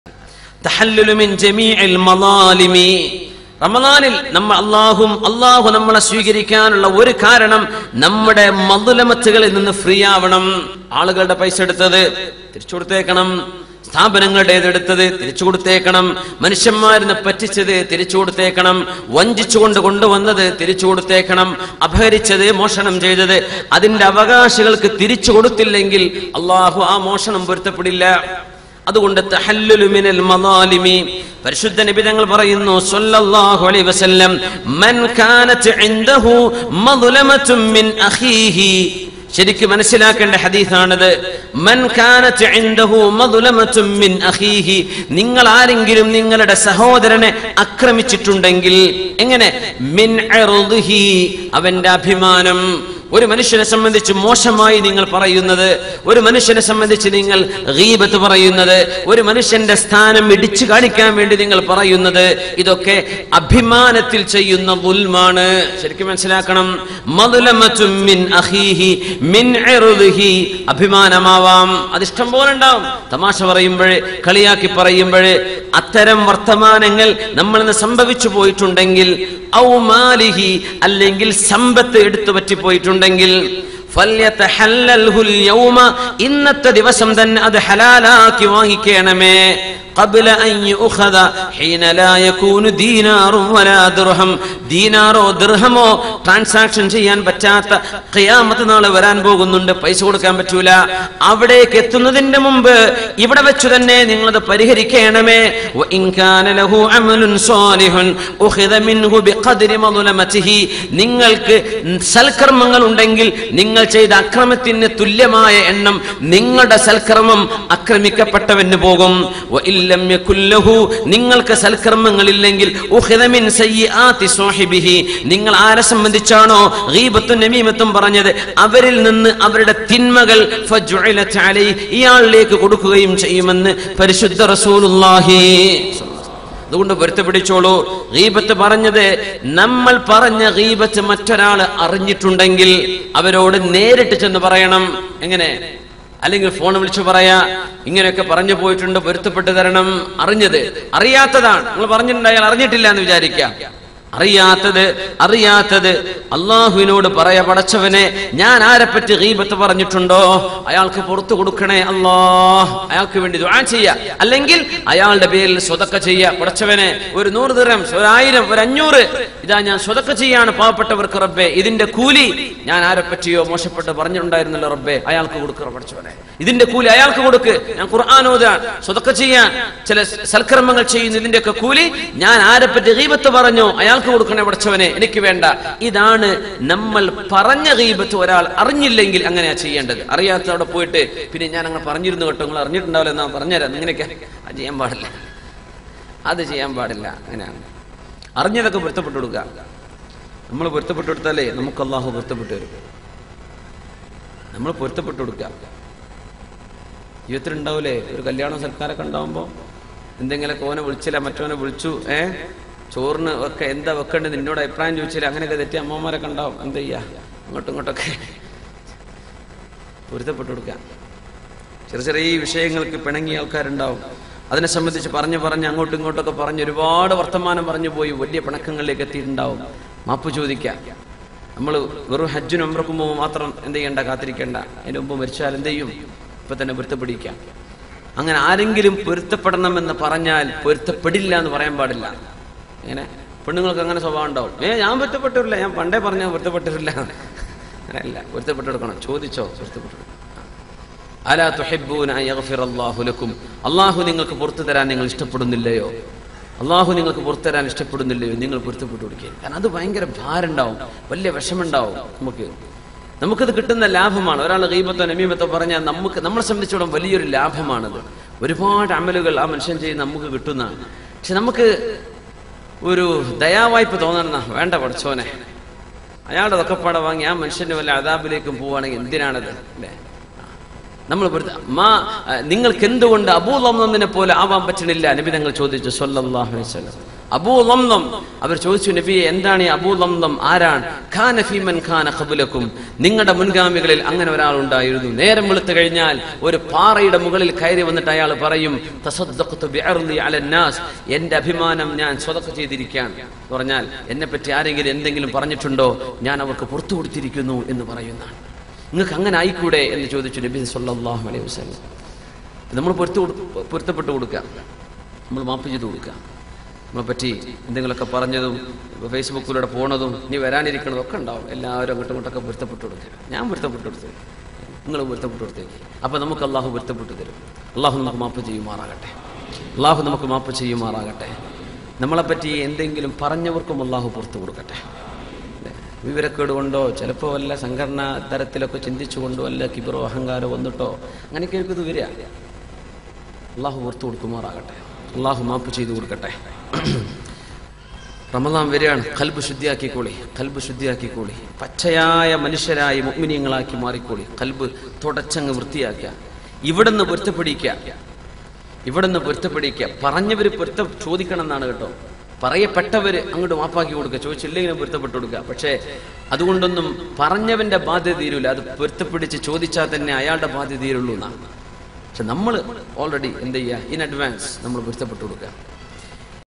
Mattullavarude Manass Vedanipichitundengil Ee Ramalanengilum Porutha Peduthuga 🎙️SIMSARUL HAQ HUDAVI
Aarengil Manass Vedanipichitundengil Porutham Kodukkuga Simsarul haq hudavi heart touching speech